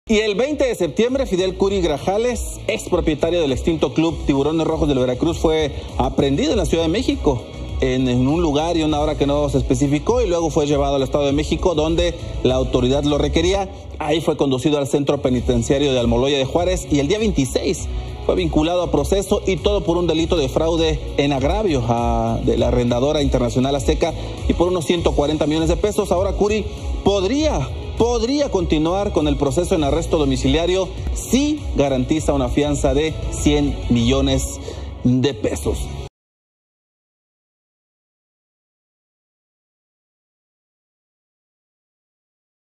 Fidel Kuri podría llevar proceso en libertad; deberá pagar fianza de 100 mdp
Una jueza del Poder Judicial del Estado de México determinó cambiar la medida cautelar de prisión preventiva del ex dueño de los Tiburones Rojos del Veracruz.